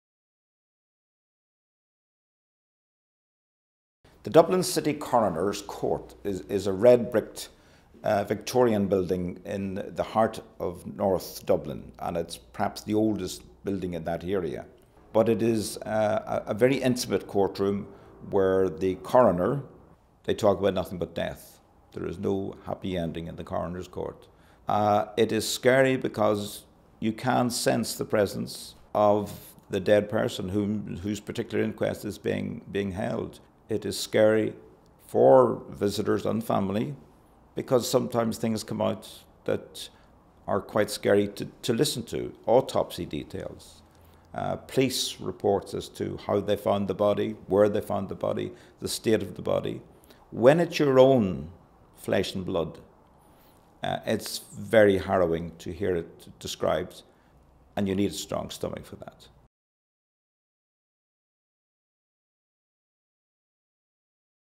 1610- Paul Carson's Book Launch - Coroner's Court
Paul Carson reveals what happens in the Coroner's Court. He talks about the horrific atmosphere in the court for the families as details about the autopsy are discussed.